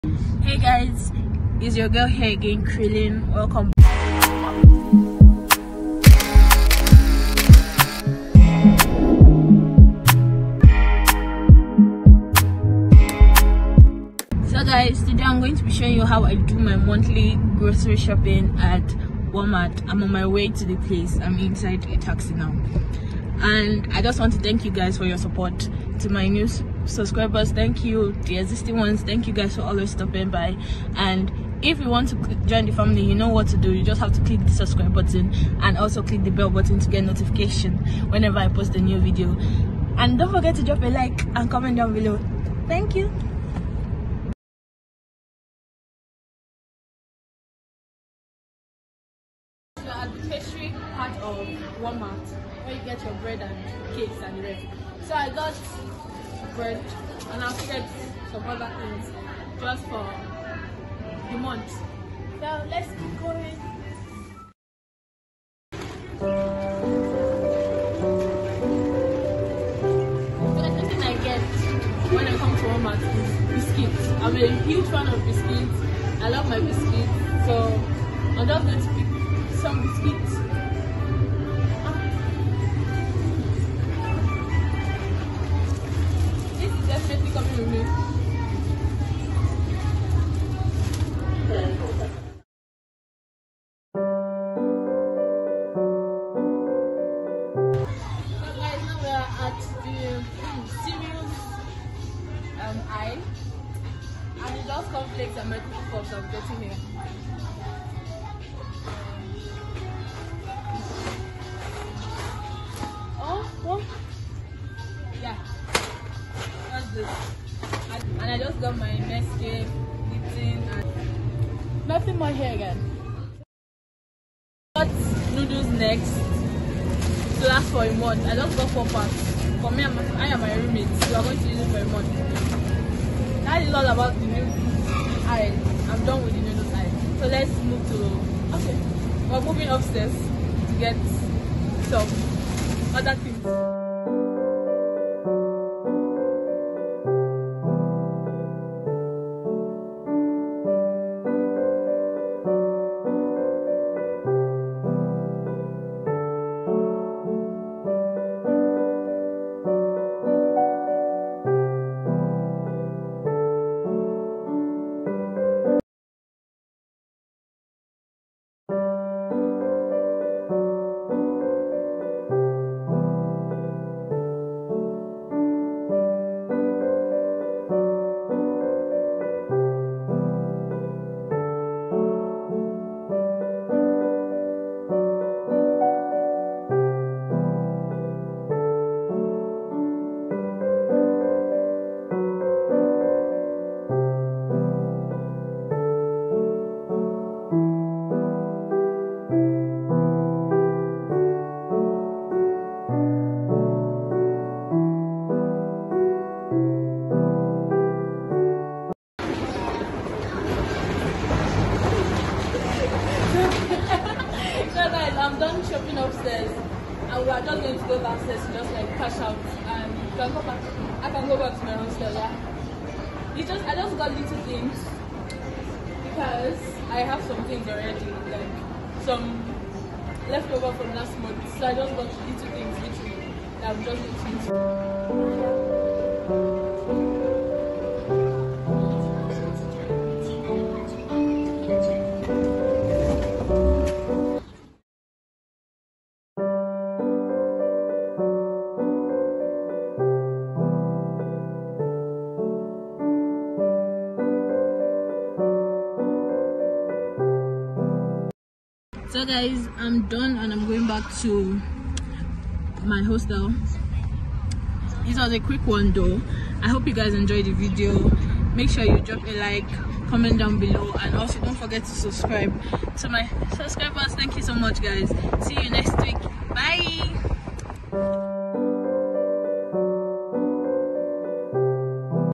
MONTHLY GROCERY SHOPPING IN CHINA || WALMART || PART 1
Hii Everyone!!!

     I made a quick video of how i shop for my groceries monthly.
  This video shows the items i will always shop for every month.
 Keep calm for Part two[2] .

Camera:IPhone 11

                IT IS FREE XOXO